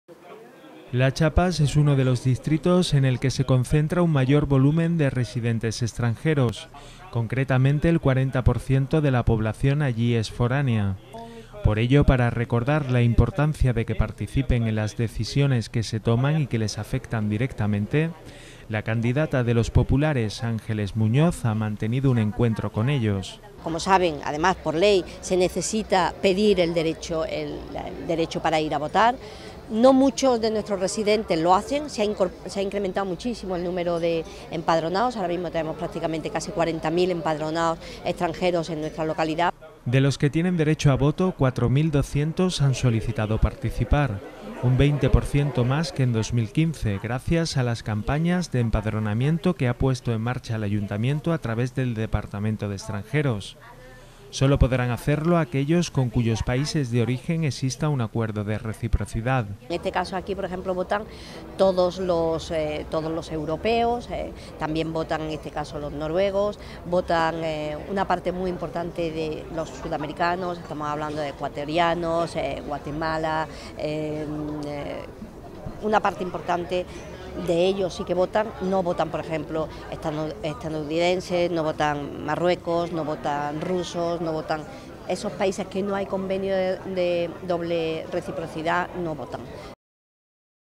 4.258 extranjeros empadronados en Marbella han solicitado ejercer su derecho al voto
Un 20 por ciento más que hace cuatro años. Sólo los extranjeros con cuyo país de origen exista un acuerdo de reciprocidad pueden hacerlo.